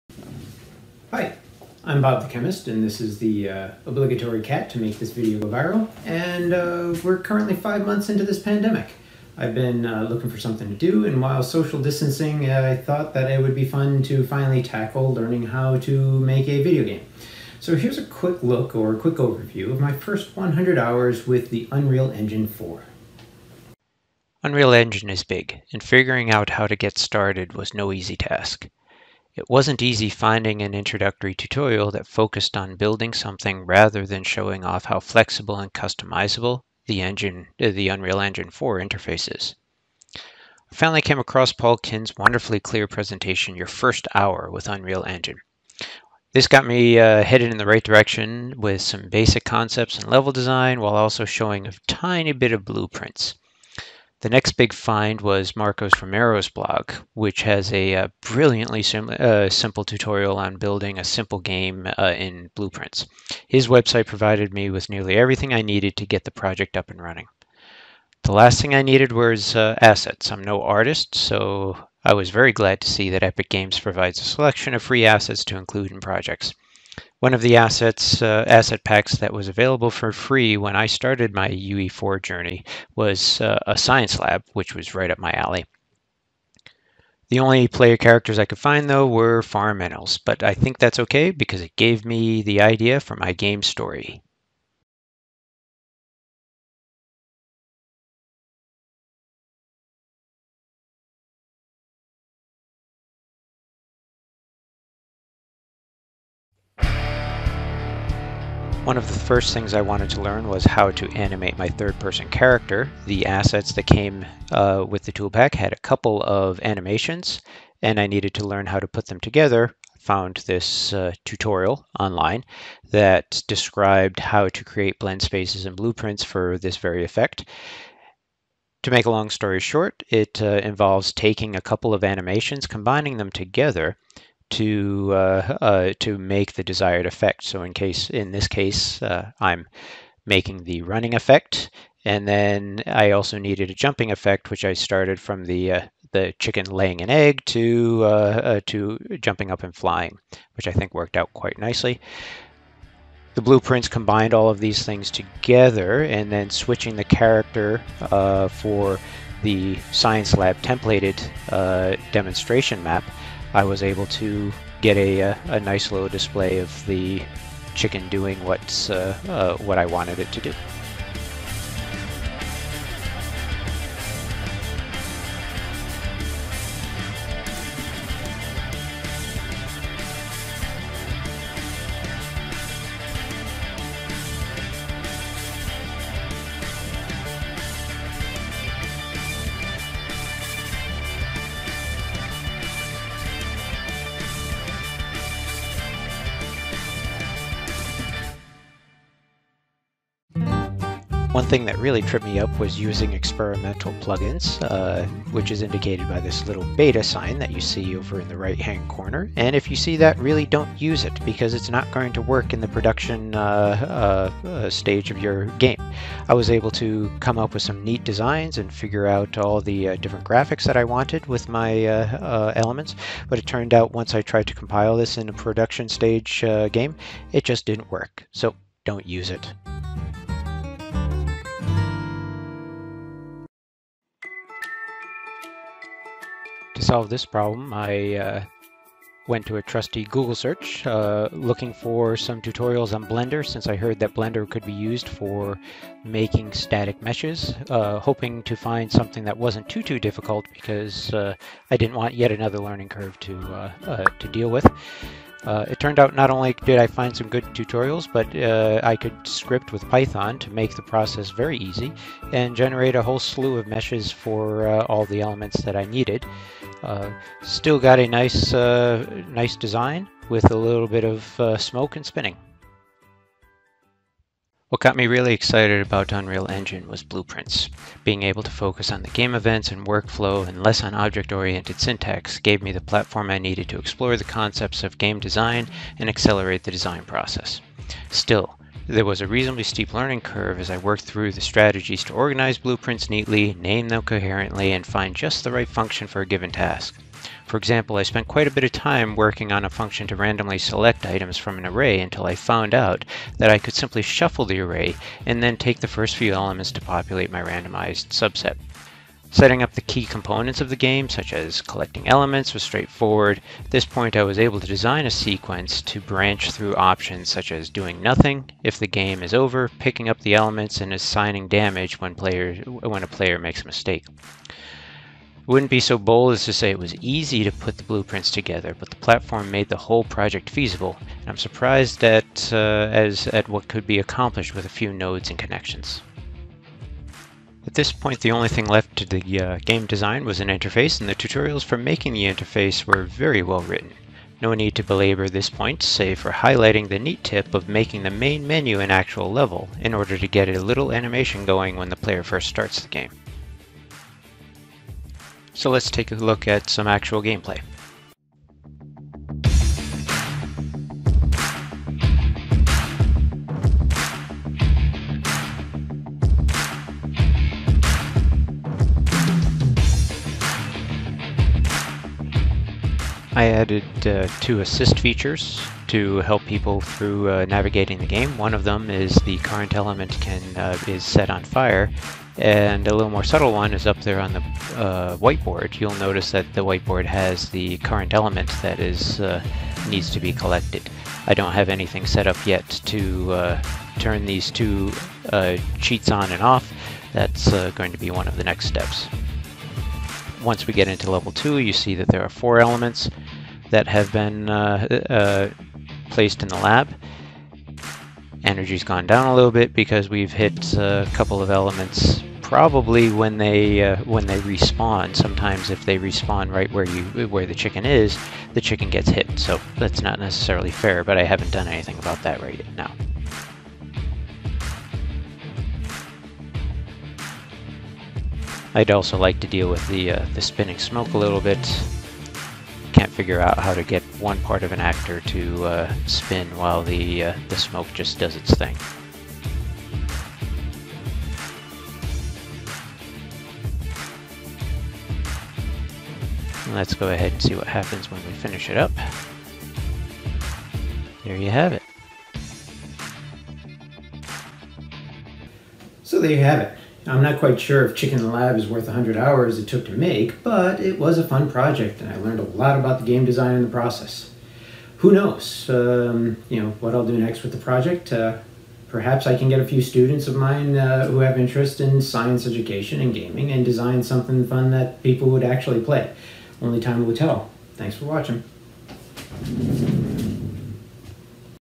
My first 100 hours with Unreal Engine 4
While trying to escape boredom in the midst of a pandemic, I wanted to learn how to make video games.  It took me about 100 hours to complete my first project using the UE4 engine.  Here's a quick overview of the project and you can learn more about it at my